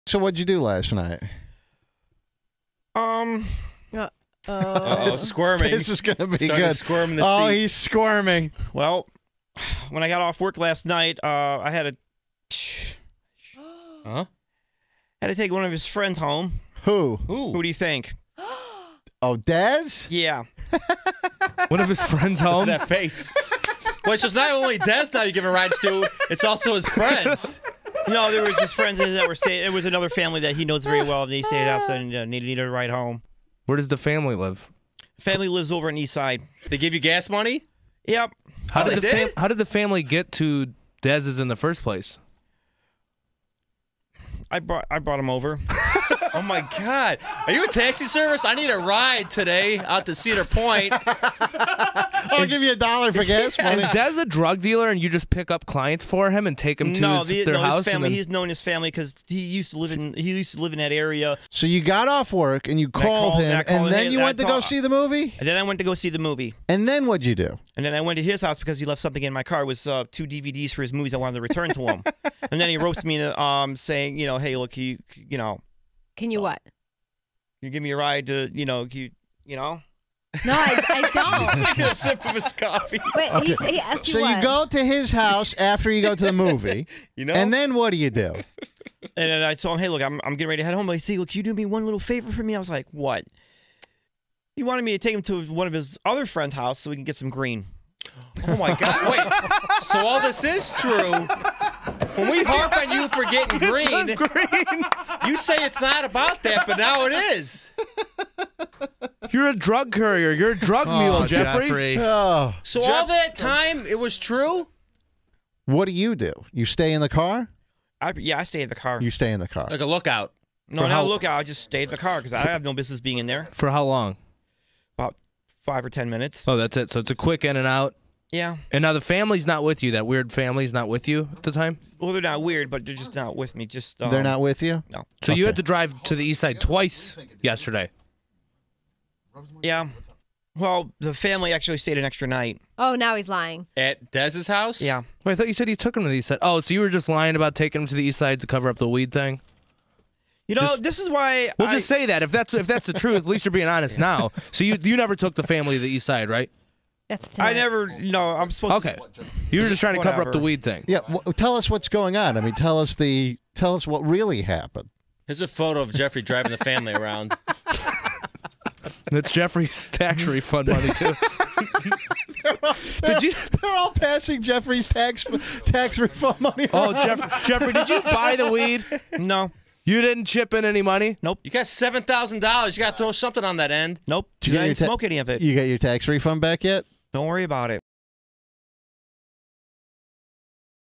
JLR Does Drug Run for His "Friend"
Jeffrey runs a personal taxi service for his "friend" - which includes drug running and driving kids with no car seats.